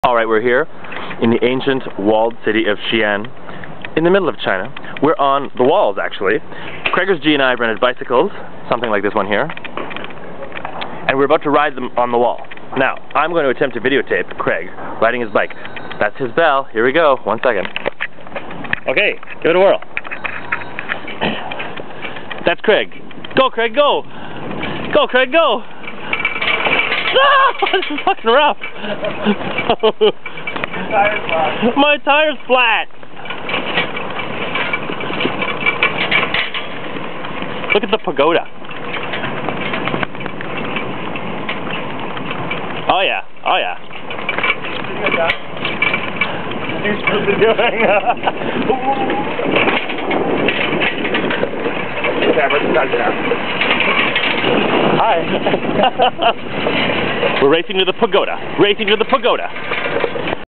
Riding bikes on the stone walls in Xian China - with a flat tire
We rented bikes and rode the whole way around the ancient walls in Xian China.  You can see how long the walls are from this clip.  They're in amazing shape for being so old.  Kind of like Quebec City - but you can't bike on the walls there.
      This is the city with the Terracotta Warriors and these walls were almost as impressive but no one stops to see the walls cuz they're all fixated on the warriors.  Silly lemmings!
  This was possibly the best bike ride ever - but man those cobble stones were tough on the crotch with a flat tire.